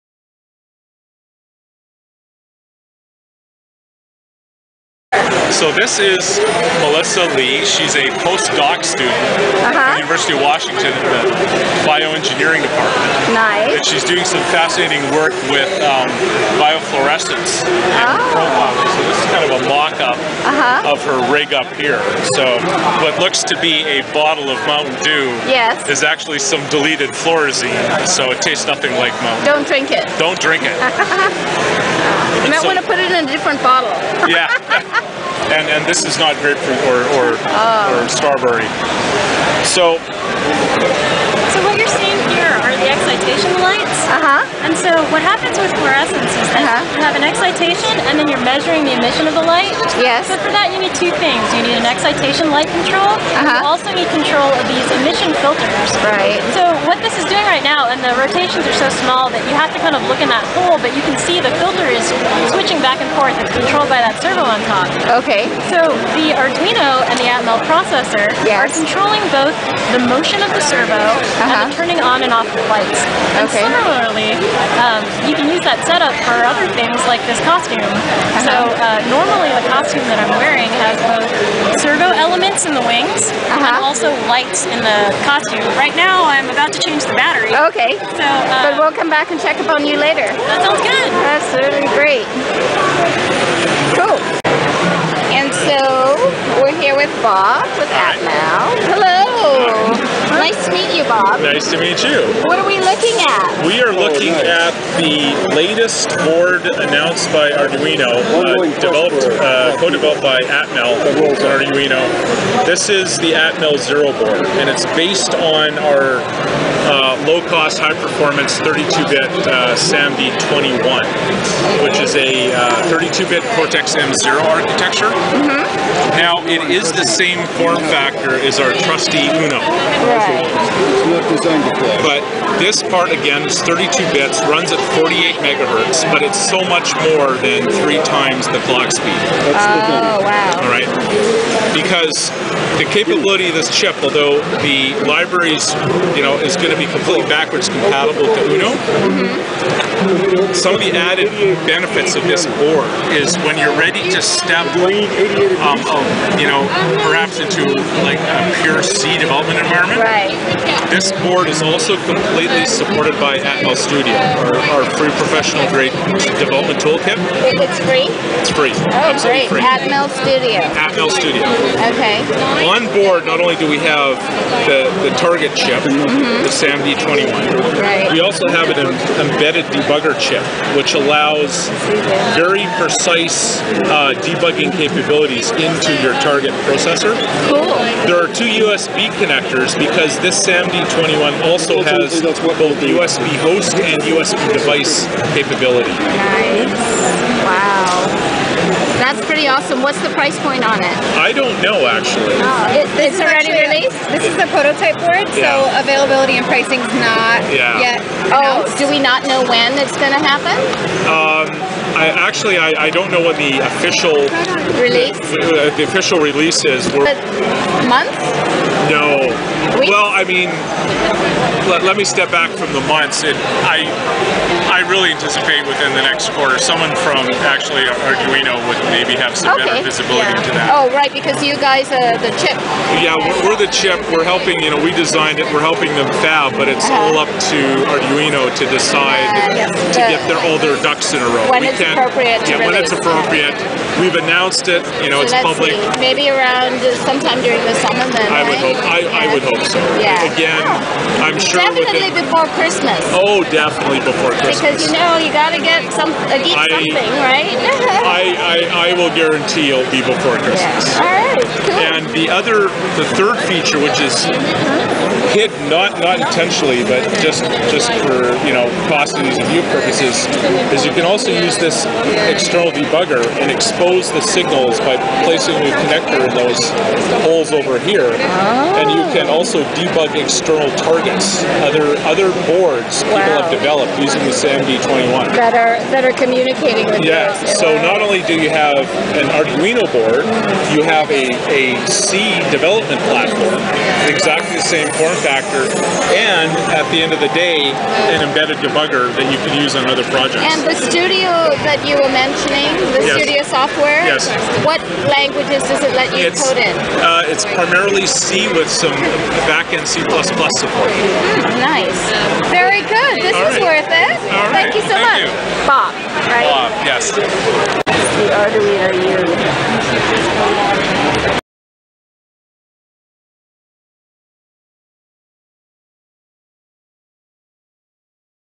Maker faire: New backward compatible 32 bit Arduino Zero - still in prototype, developed by Atmel
It's compatible with shields made for the Arduino Uno R3, and is compatible with their studio development software. (C, C++)  At the Bay Area Maker faire.